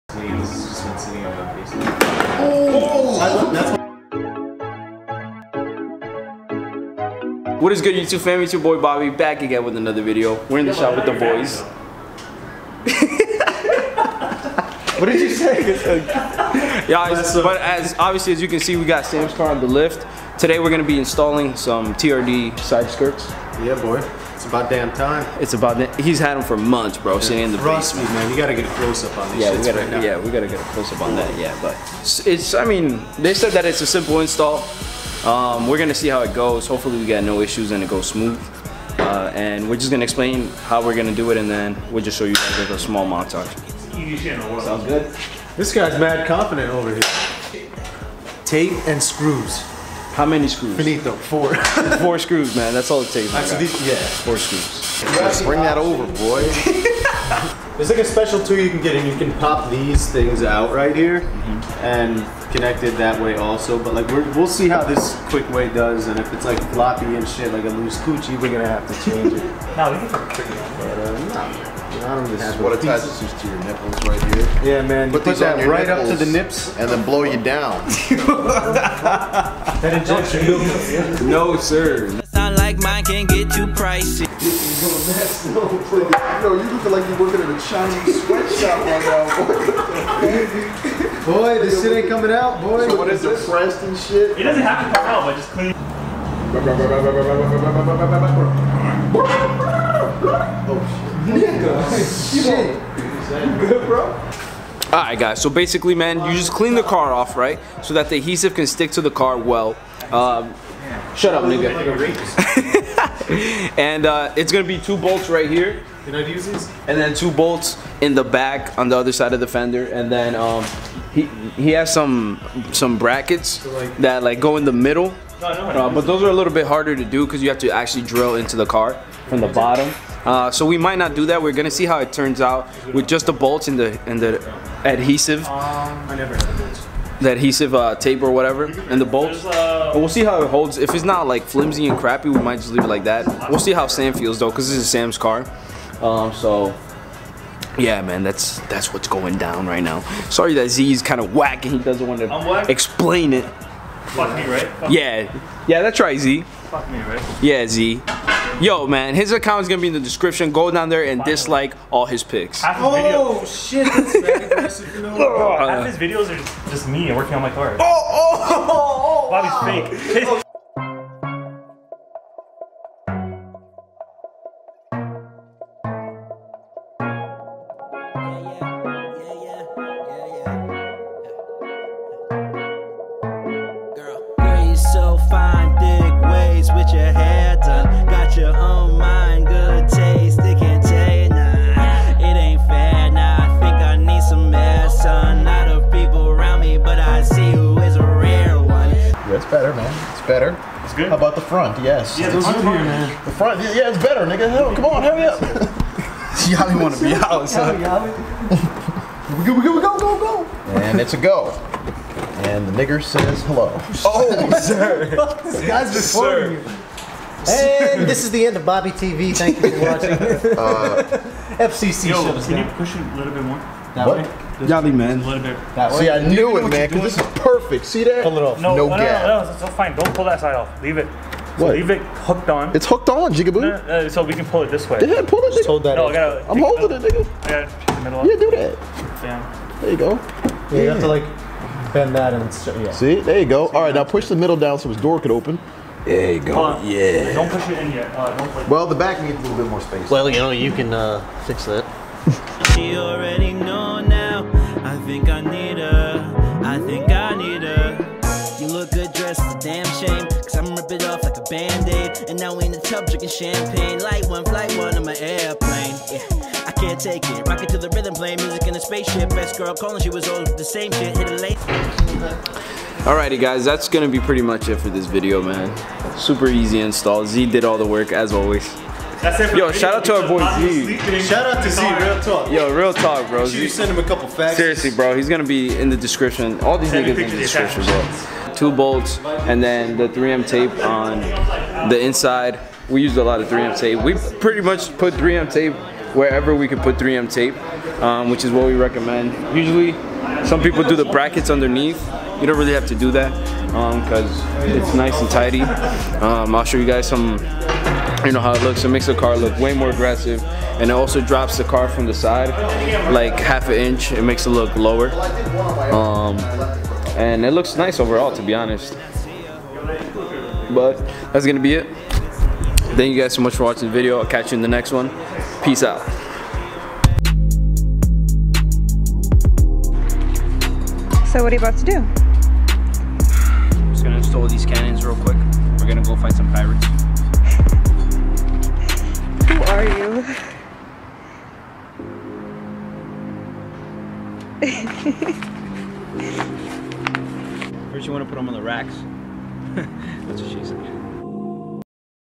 How To Install Sideskirts ! (BRZ/FRS/GT86)
Hope this video was somewhat of help to anyone trying to install these! It’s a pretty simple and straight forward install! Very easy and requires almost no skil! Thank you so much for the support !